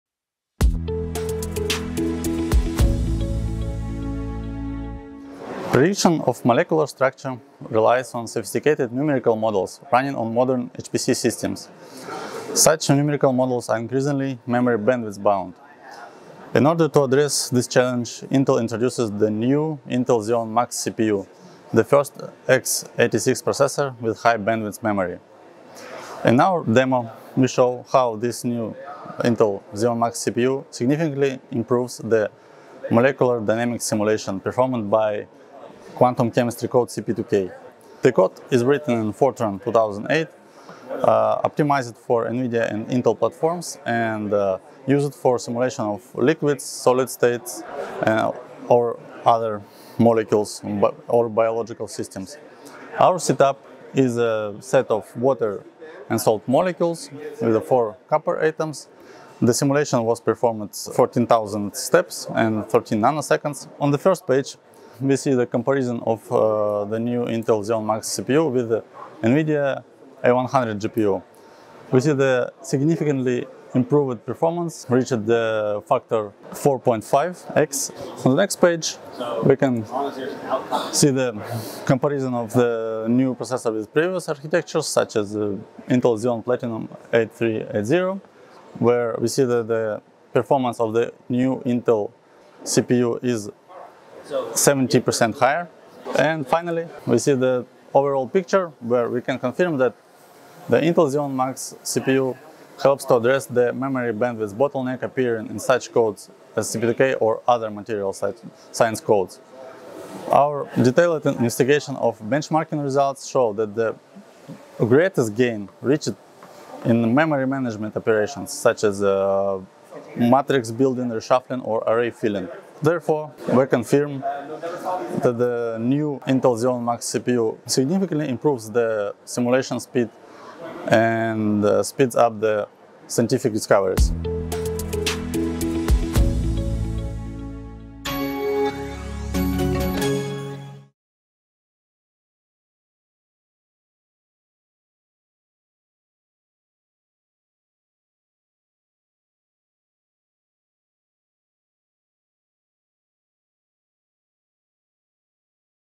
Speedup Quantum Chemistry Simulations | Intel Business
This is a demo capture from the ISC23 event that shows leadership performance of Intel Xeon Max CPU  vs. NVIDIA A100 GPU when running a molecular dynamics simulation code.